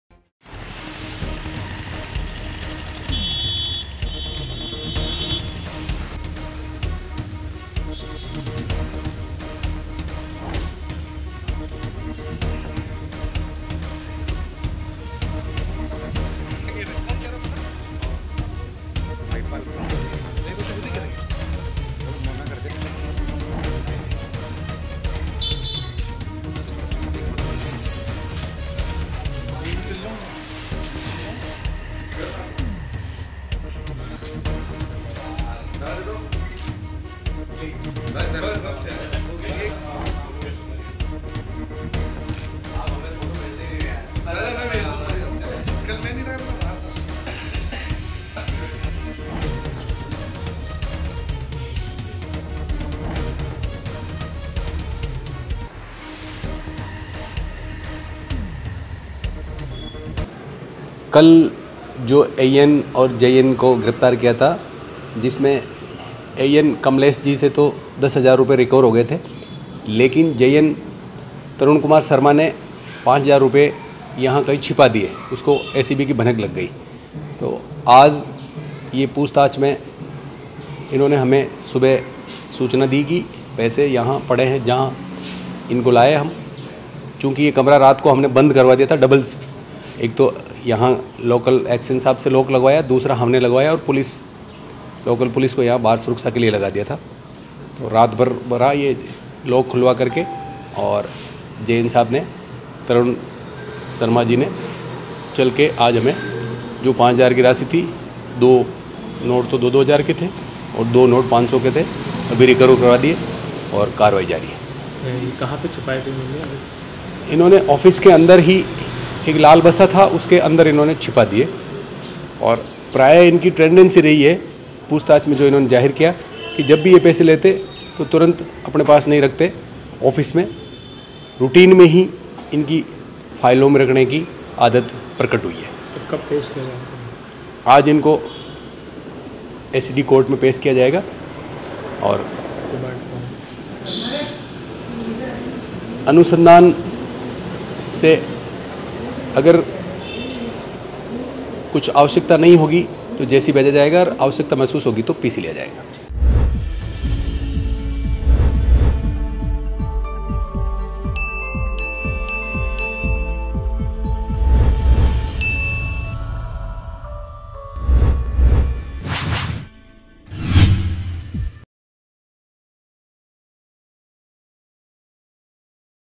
AJMER NEWS | ACB द्वारा जलदाय विभाग के AEN,JEN ट्रैप मामला | MTTV INDIA
ACB द्वारा जलदाय विभाग के AEN,JEN ट्रैप मामला

जेईएन द्वारा फेंके गए 5000 रुपए मिले सहायक अभियंता कमलेश मीणा के चेंबर में

अजमेर ACBकर रही मामले में जेईएन तरुण कुमार से पूछताछ

लोहा खान स्थित जलदाय विभाग में हुई थी ट्रैप की कार्रवाई